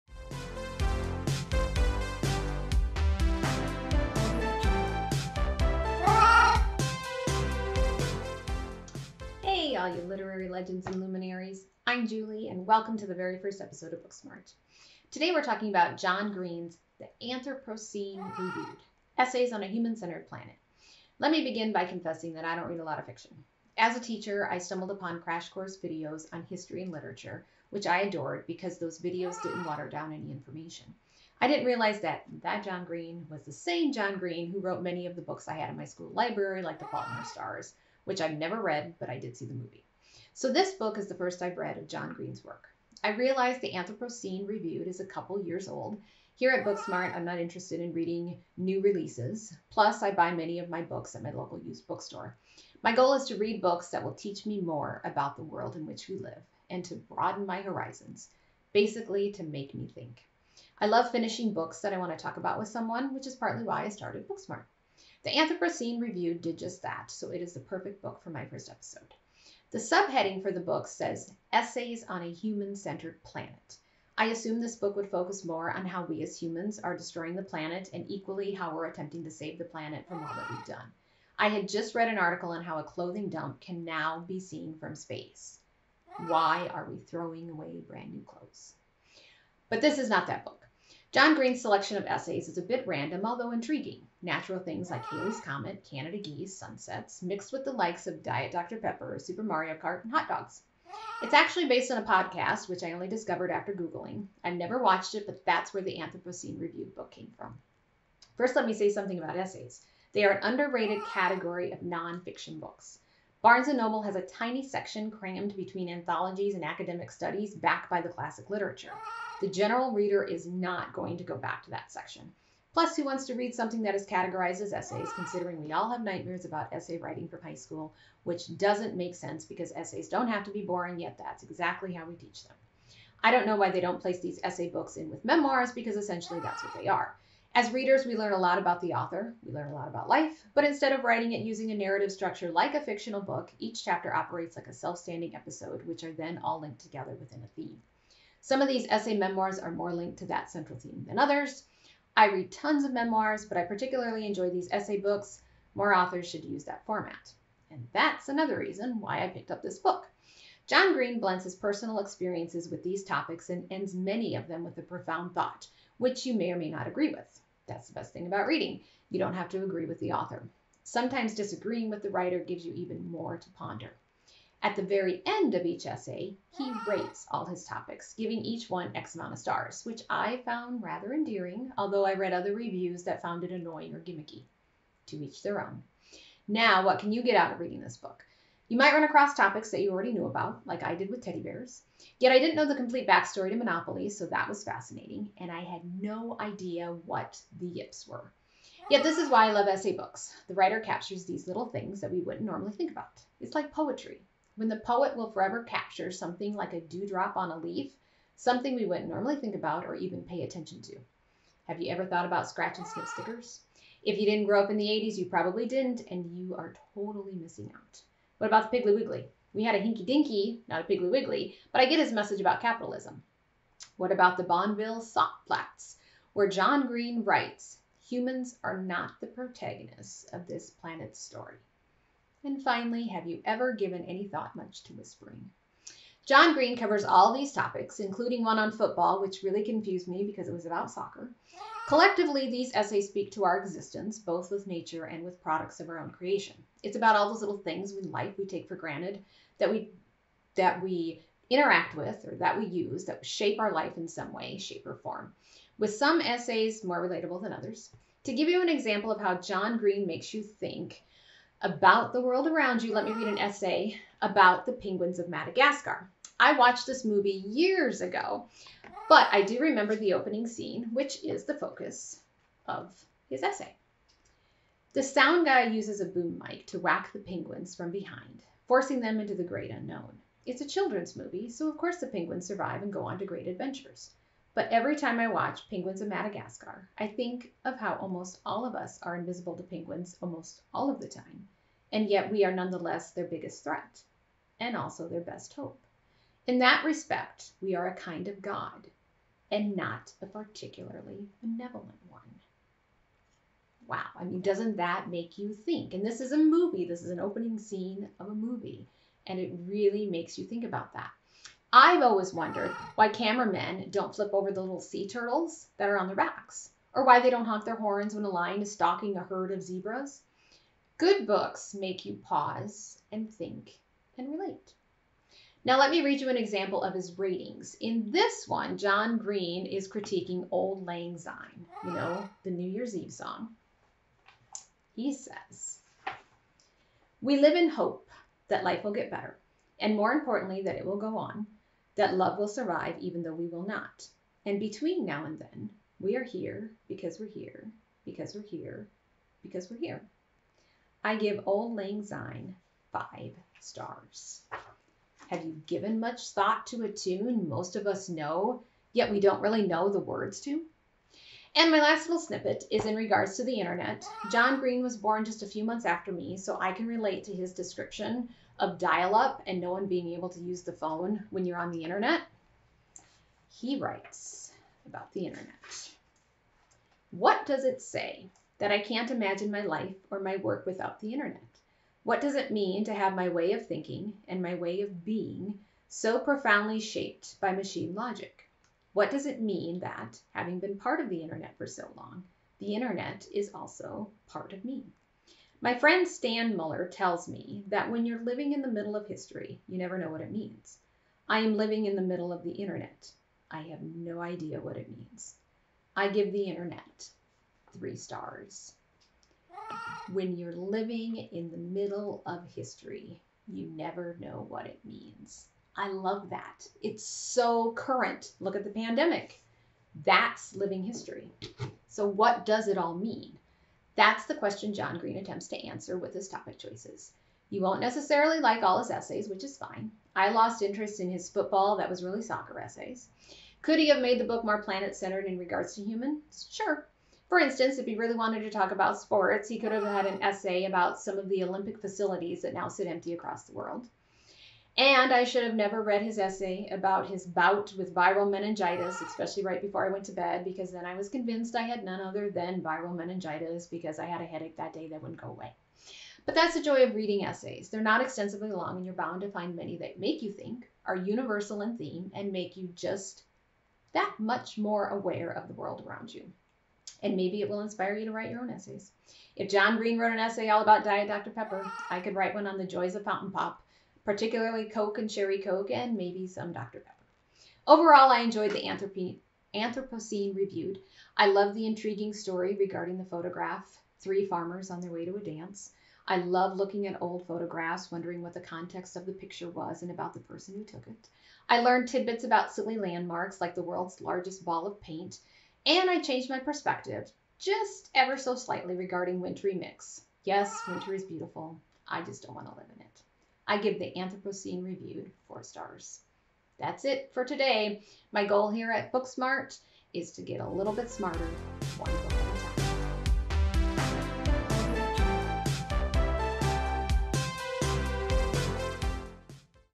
The Anthropocene Reviewed
At Book Smart, learn fun facts through popups as I review John Green's The Anthropocene Reviewed.  I review two books a week–primarily non-fiction–with the ultimate goal of getting a little bit smarter, one book at a time.